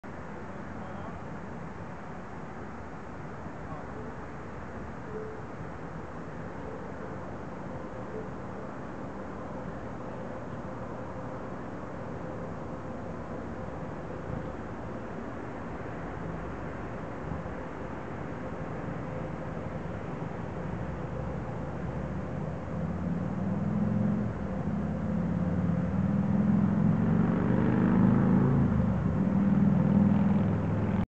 Off The Lot
Ana driving her new car out of the car lot.